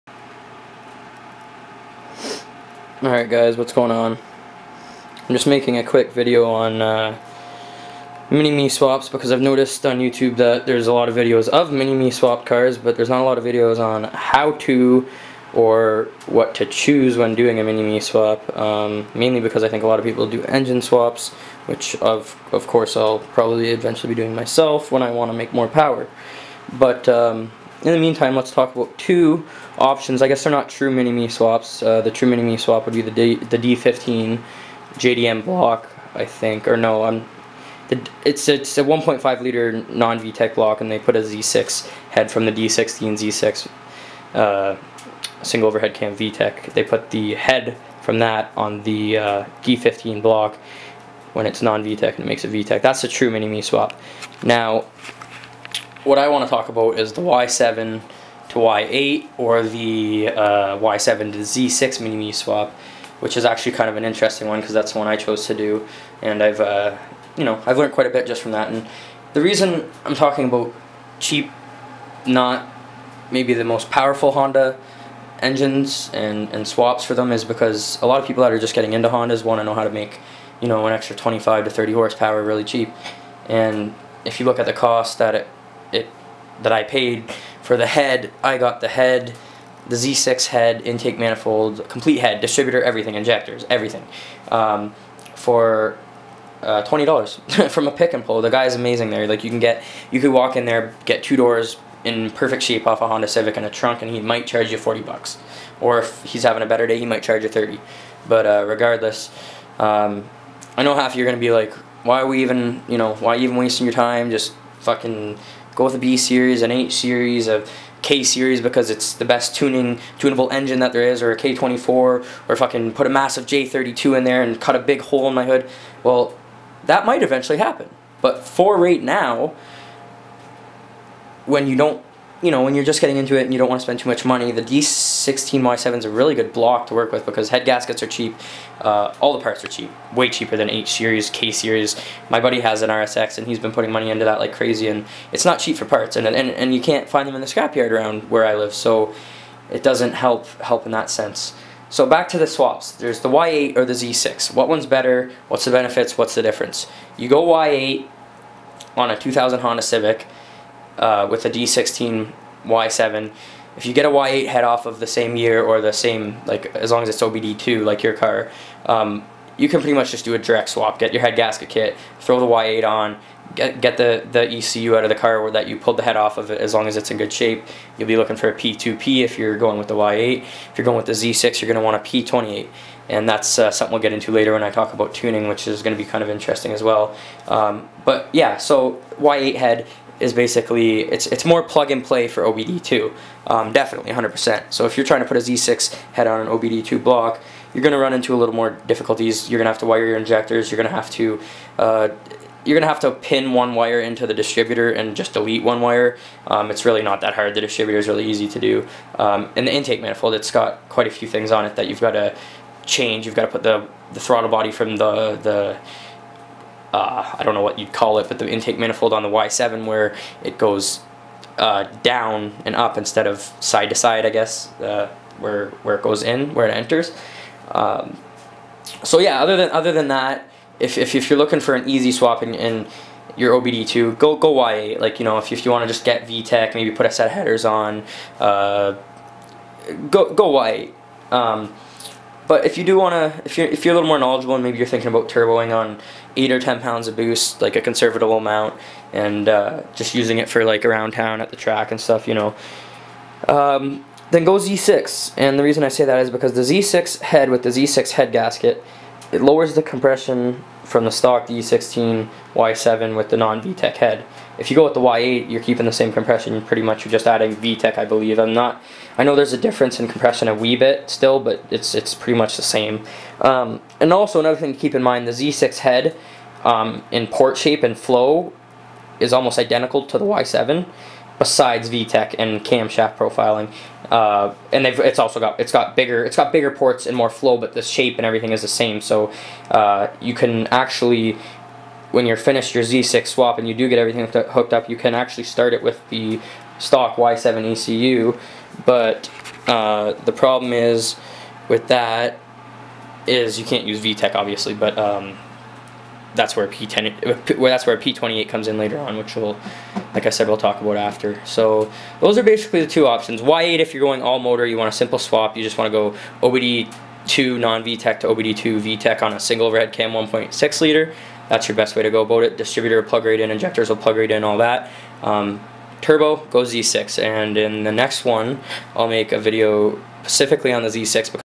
Mini me swap (y8 or z6 on a "d16y7" block)
Just a little info on z6 and y8 head swaps, to help you choose what's best for you're setup.